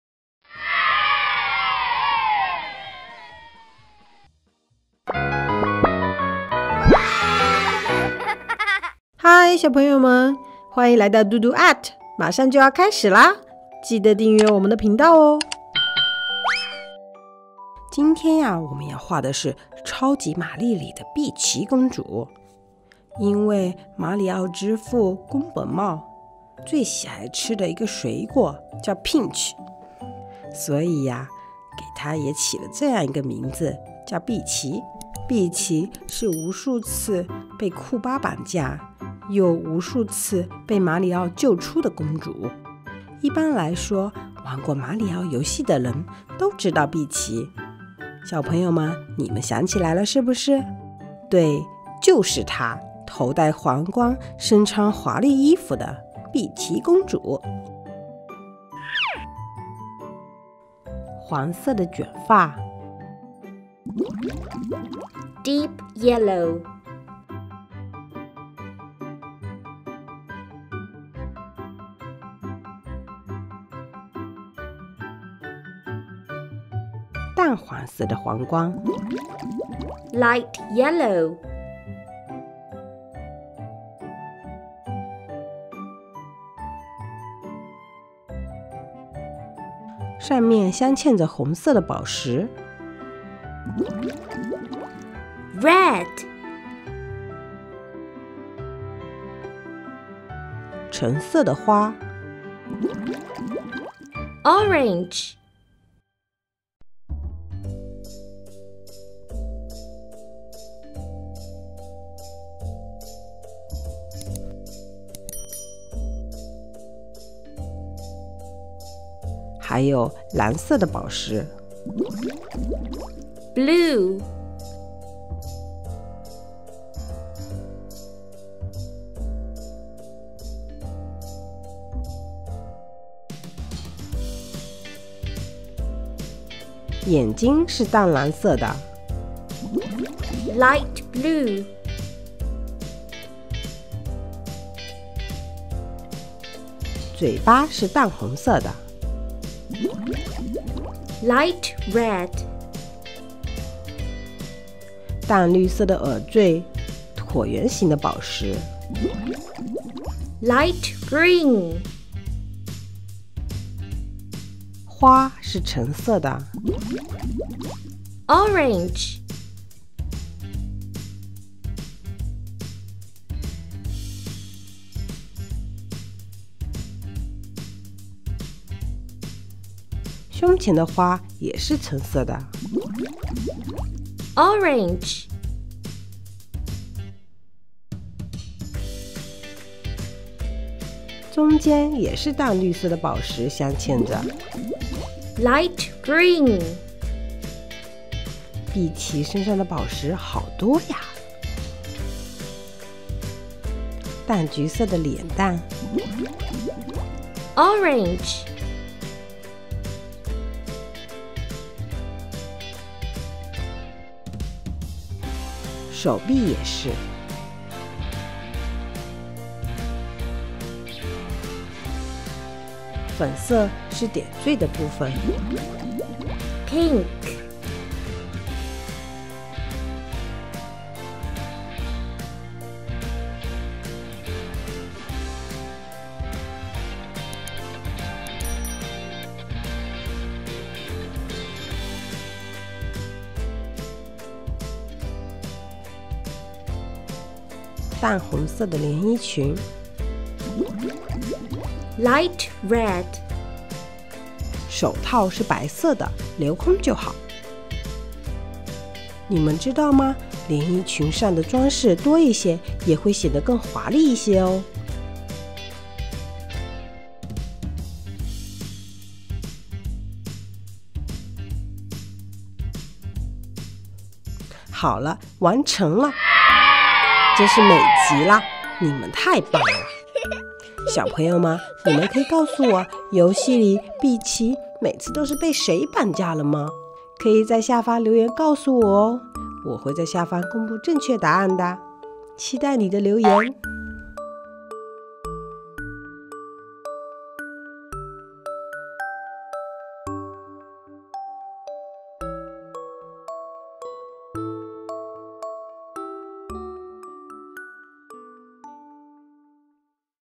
簡易繪圖｜迪士尼公主系列（二）碧琪公主｜學顏色認知｜每天一點簡筆畫｜家長必須觀看的兒童繪畫啟蒙課程｜早教課堂
2-4歲是小朋友形成顏色認知的關鍵時刻，
跟著Dudu的早教簡筆畫課程，專業的課程設計，
讓妳的小孩動手動腦，學習顏色，快樂成長!